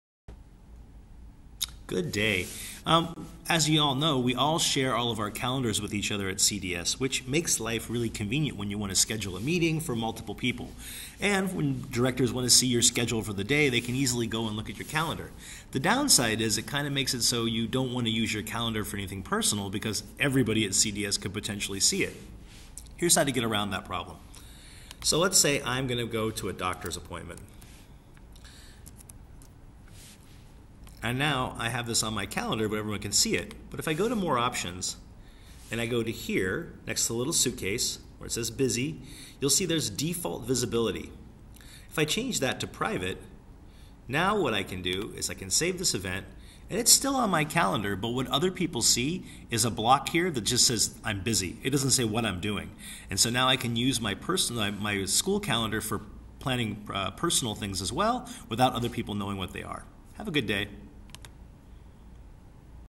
Making a Calendar Event Private
How to create a private Google Calendar event on your public calendar.